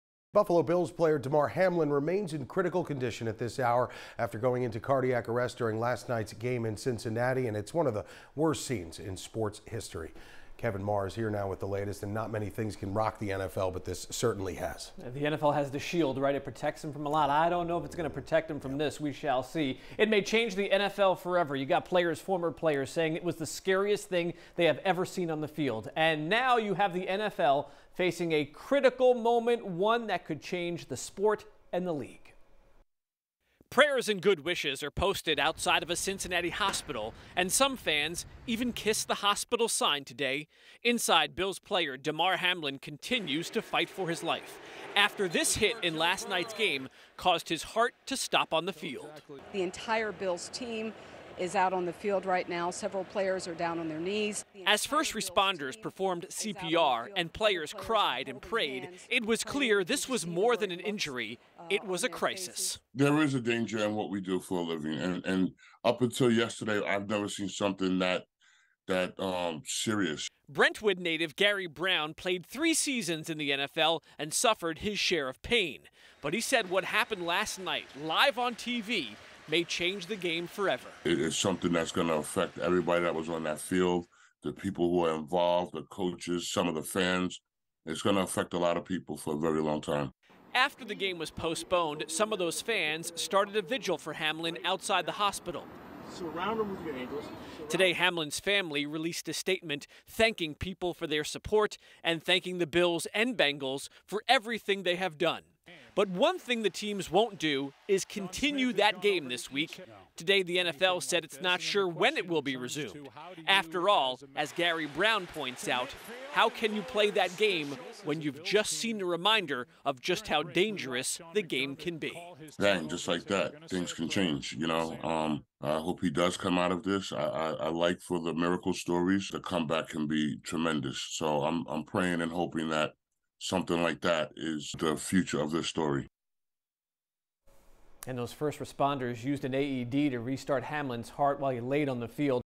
Bills' Hamlin in critical condition after collapse on field
Buffalo defensive back Damar Hamlin collapsed on the field during the first quarter of the nationally televised game against the Cincinnati Bengals and was given medical treatment for nearly 20 minutes before being taken to a hospital.